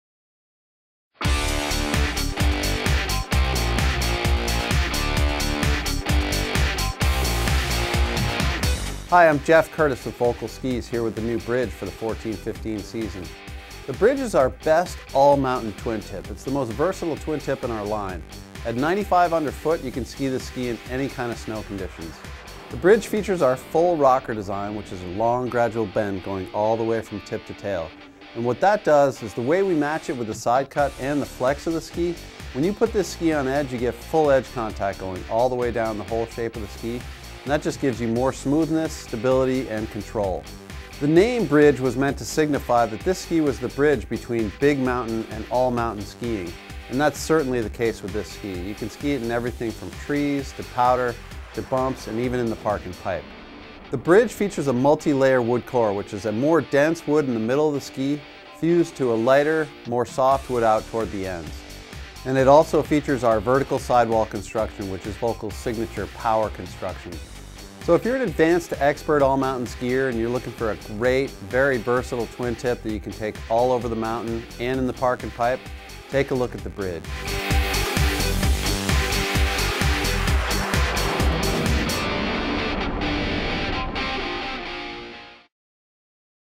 Völkl Bridge 1415
The Bridge features a Full Rocker-specific shape of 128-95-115, with a smooth, gradual reverse camber, allowing deep snow maneuverability along with solid groomed snow performance. It’s a smooth, playful ride for the ultimate all mountain experience, ranging from big mountain conditions all the way to the park.